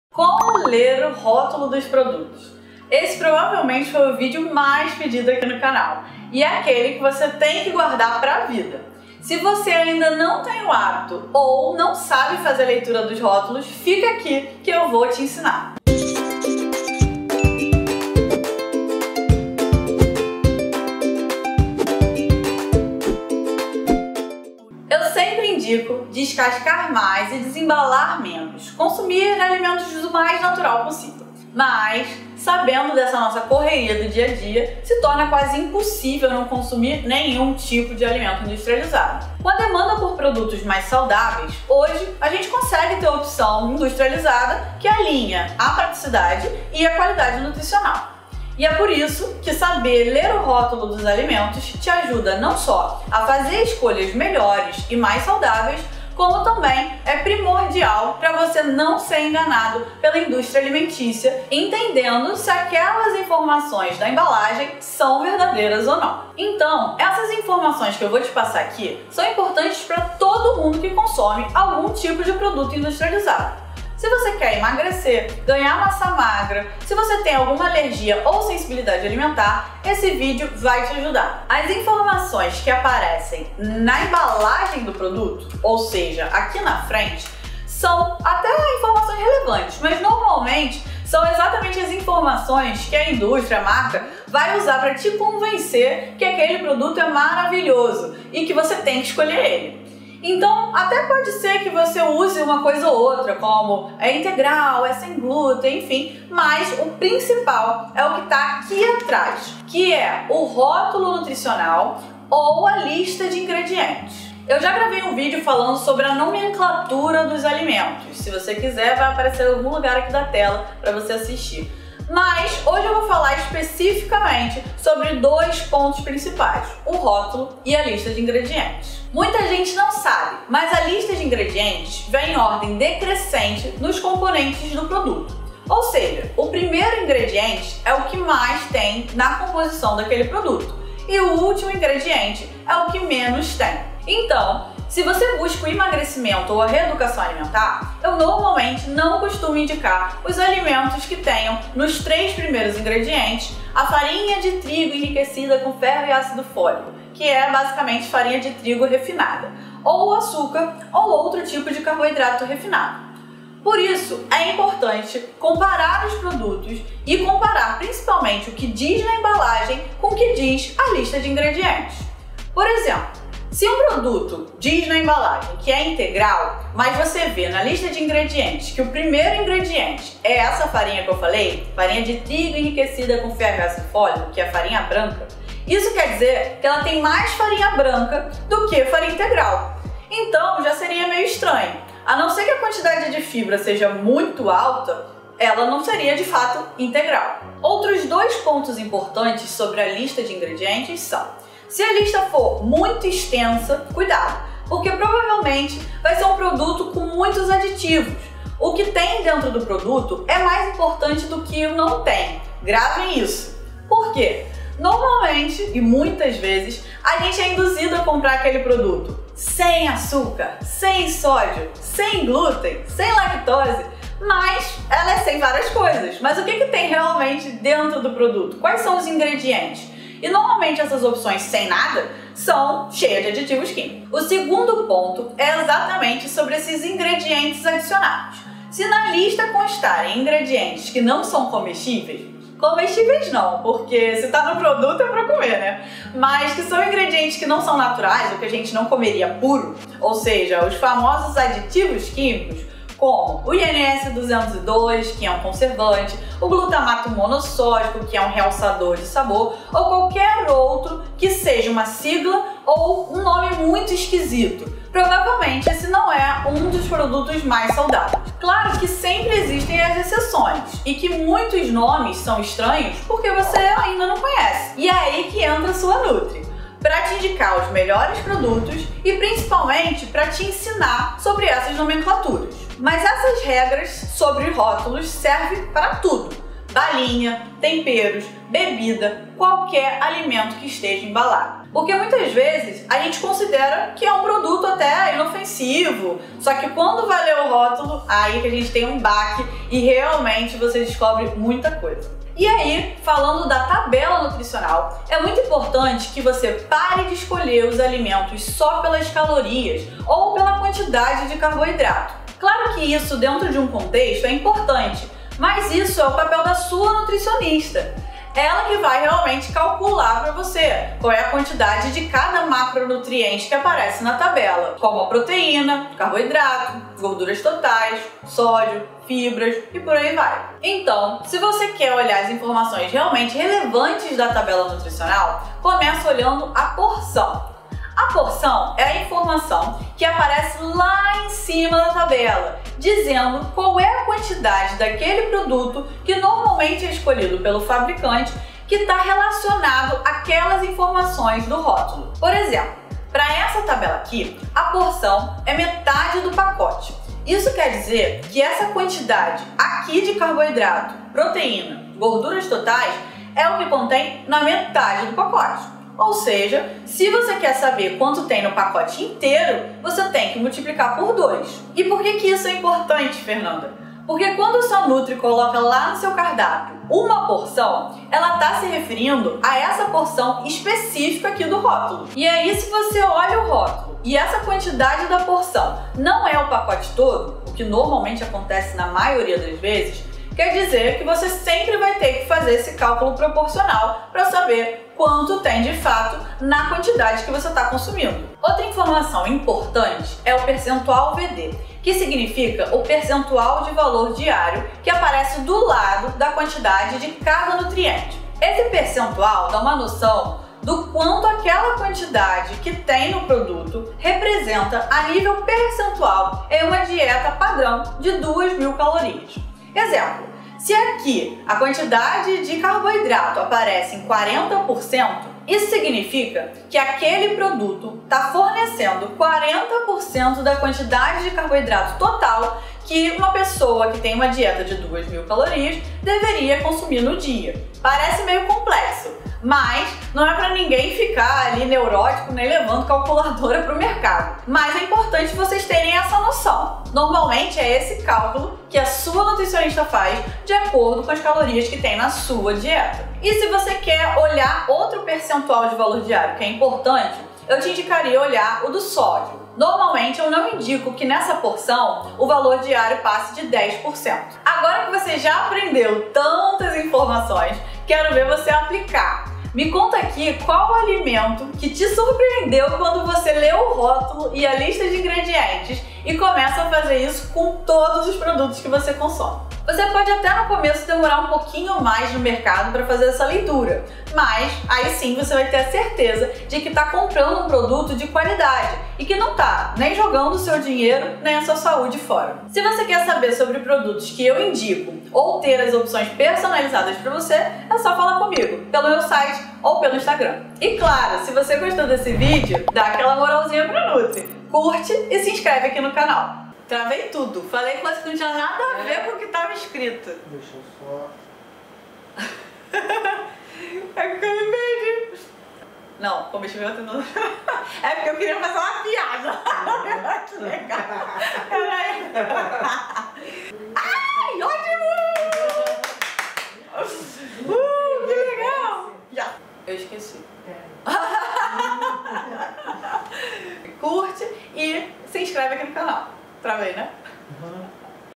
Saiba ler o rótulo dos produtos - Nutricionista Fernanda Marques
Um dos vídeos mais pedidos no canal!
Saiba como interpretar os rótulos de uma maneira simples e muito funcional.

Lá no meu instagram dou ainda mais dicas sobre esse assuntos e muitos outros. Me siga para mais conteúdo: